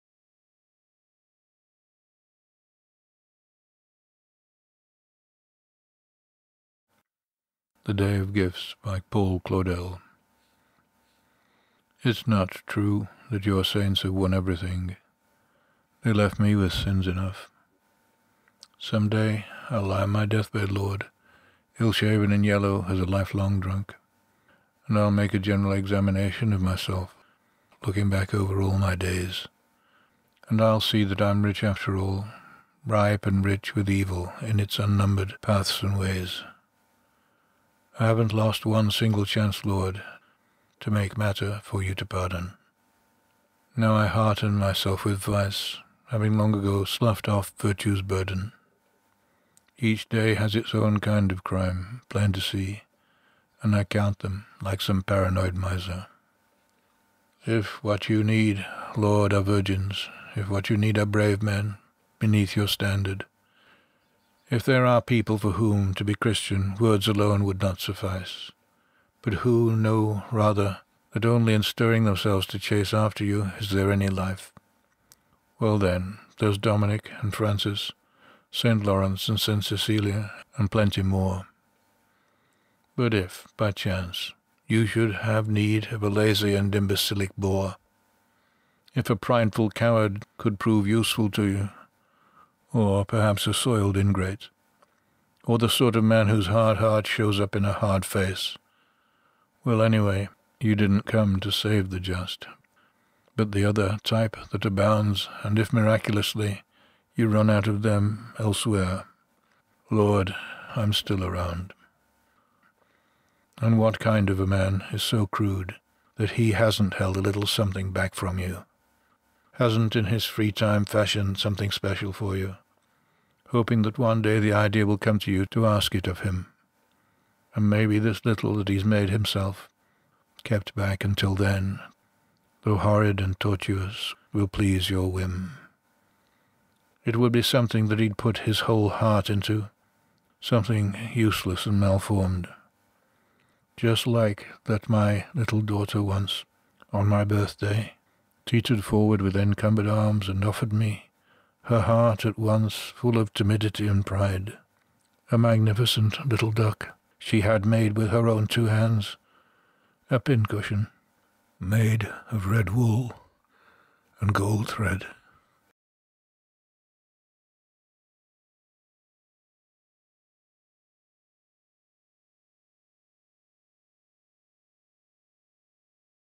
The Day of Gifts by Paul Claudel (read by Tom O'Bedlam)
"Claudel wrote in a unique verse style. He rejected traditional metrics in favour of long, luxuriant, unrhymed lines of free verse, the so-called verset claudelien, influenced by the Latin psalms of the Vulgate."

Christopher Hitchens' essay, Love, Poverty, and War, quotes "In memory of WB Yeats" by W H Auden

Time that is intolerant
Of the brave and the innocent,
And indifferent in a week
To a beautiful physique,

Worships language and forgives
Everyone by whom it lives;
Pardons cowardice, conceit,
Lays its honors at their feet.

Time that with this strange excuse
Pardoned Kipling and his views,
And will pardon Paul Claudel,
Pardons him for writing well.

It's often misquoted and misunderstood.  The "him" referred to in the last line is Yeats, not Claudel.
I guess that Hitchens identified with this, hoping that time would pardon him too for writing well.

Franz Joseph on his deathbed, oil painting, 1916 by Franz von Matsch.
Little girl holding apples in her hands 1895 William Bouguereau

It's not true that Your saints have won everything: they left me with sins enough.
Someday I'll lie on my deathbed, Lord, ill-shaven and yellow as a lifelong drunk.
And I' ll make a general examination of myself, looking back over all my days,
And I'll see that I'm rich after all, ripe and rich with evil in its unnumbered paths and ways.
I haven't lost one single chance, Lord, to make matter for You to pardon.
Now I hearten myself with vice, having long ago sloughed off virtue's burden.
Each day has its own kind of crime, plain to see, and I count them like some paranoid miser.

If what you need, Lord, are virgins, if what you need are brave men beneath your standard;
If there are people for whom to be Christian words alone would not suffice,
But who know rather that only in stirring themselves to chase after You is there any life,
Well then there's Dominic and Francis, Saint Lawrence and Saint Cecilia and plenty more!
But if by chance You should have need of a lazy and imbecilic bore,
If a prideful coward could prove useful to You, or perhaps a soiled ingrate,
Or the sort of man whose hard heart shows up in a hard face—
Well, anyway, You didn't come to save the just but the other type that abounds,
And if, miraculously, You run out of them elsewhere . . . Lord, I'm still around.

And what kind of a man is so crude that he hasn't held a little something back from You,
Hasn't in his free time fashioned something special for You,
Hoping that one day the idea will come to You to ask it of him,
And maybe this little that he's made himself, kept back until then,
though horrid and tortuous, will please Your whim.
It would be something that he'd put his whole heart into, something useless and malformed.
Just like that my little daughter once, on my birthday, teetered forward with encumbered arms
And offered me, her heart at once full of timidity and pride,
A magnificent little duck  she had made with her own two hands,
a pincushion, made of red wool and gold thread.